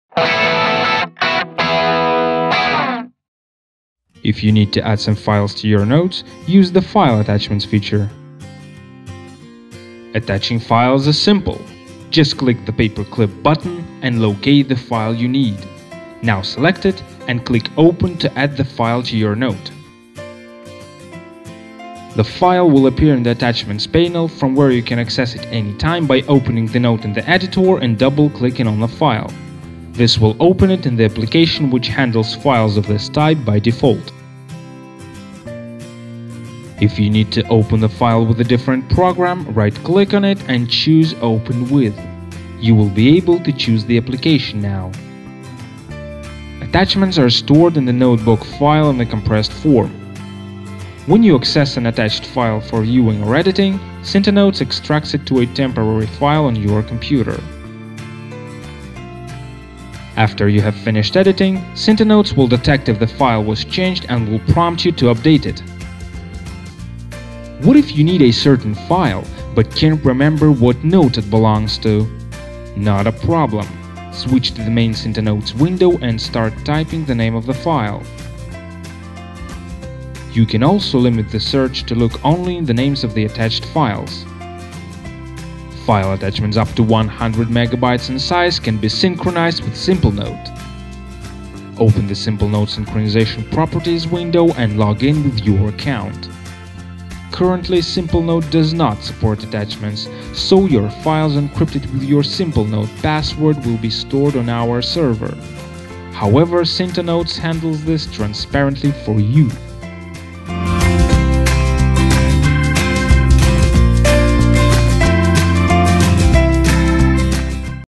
File Attachments in CintaNotes
CintaNotes 3.0 with File Attachments Released!
We've got great news for you - the long awaited file attachments feature is finally here!
The main point is simple: now you can attach files to notes, and even sync them between your computers.
The files are stored in the compressed form in the notebook itself. So when you attach a 10 MB file, the size of the notebook may grow by much less than 10 MB (this depends on the type of the file - if it's already compressed, the notebook will grow by the whole 10 MB and maybe even a little bit more). On average, however, the benefit of compression is noticeable.
The total size of attachments should not exceed 4 GB, and size of a single attachment should not exceed 2 GB. For attachment syncing, however, we had to put a much stricter limit - 100 MB, because our own server space will be used for storing uploaded attachments (you might remember this story: we were halfway though developing the feature when it turned out that Simplenote didn't have any support for file storage and transfer). In future the server storage space is likely to be increased.
All being well and good, I also have to warn you that image-related functions are now quite basic. Some improvements are planned for upcoming versions, and some other are now only considered:
As of now, there are no images appearing directly in text (inline images), and there are no image thumbnails as well. Images appear as attached files. In upcoming versions, we'll definitely make better previews for images, so that you don't have to double-click on them to view them.
To compensate for the lack of inline images, we plan to add ability to insert links to attached files into text.
Pasting images from clipboard and clipping images from webpages is planned for next versions.
Hope you enjoy the new version!